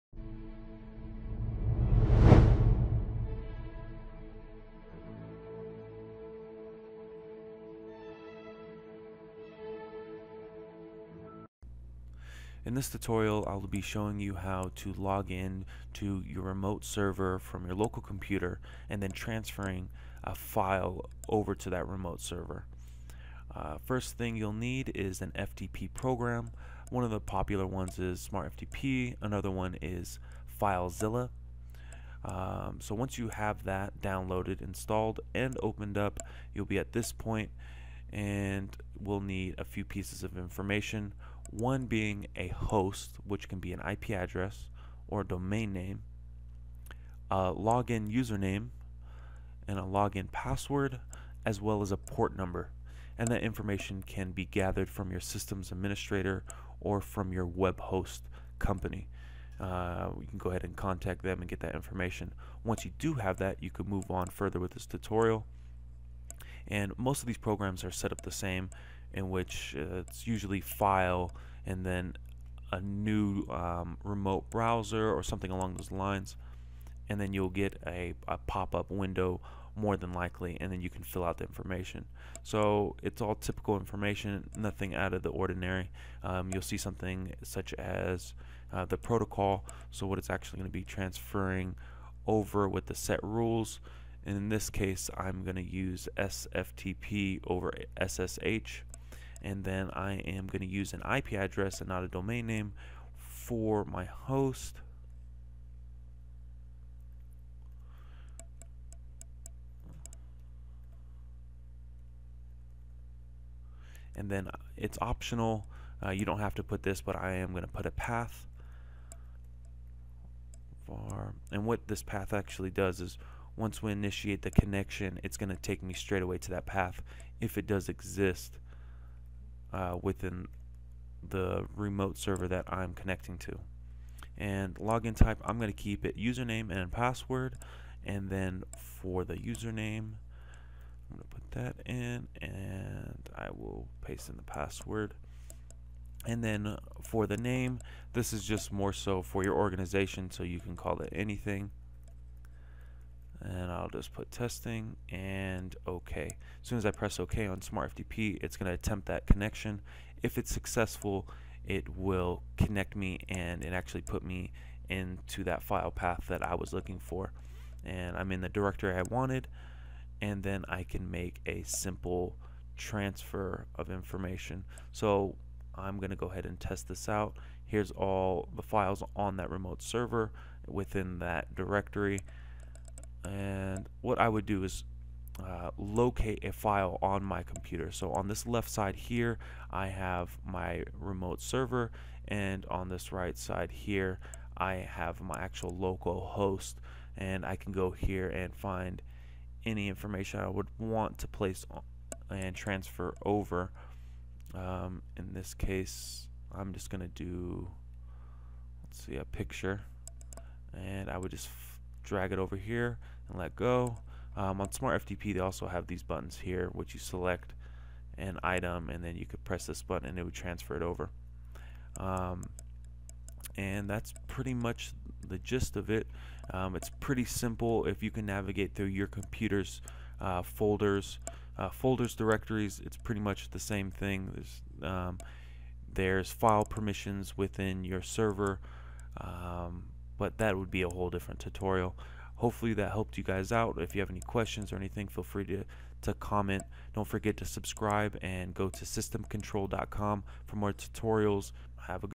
How to connect to a server and transfer files using FTP
Learn how to connect to a remote machine and transfer files using FTP.